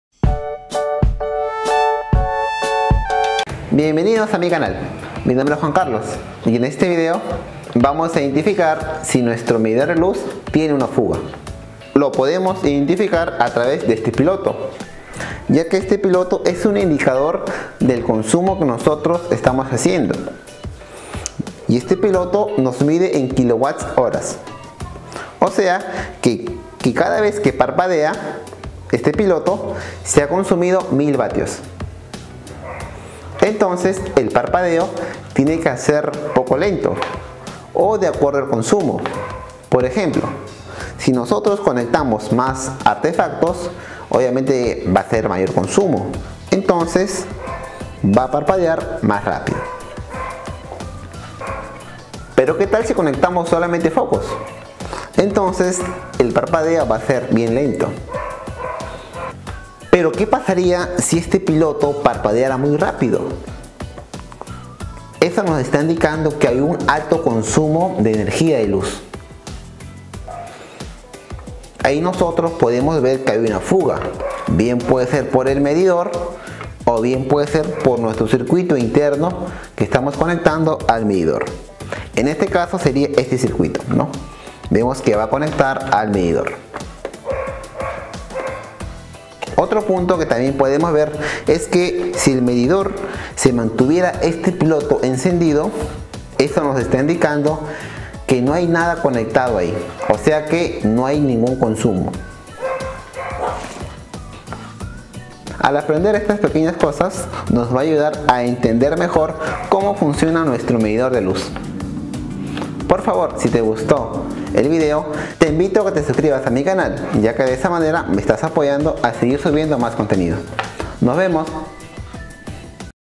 Como detectar si un Medidor de luz tiene Fuga de corriente
En este vídeo aprenderás a conocer las posibles fugas que puede pasar, también te ayudara a saber como funciona tu medidor de luz.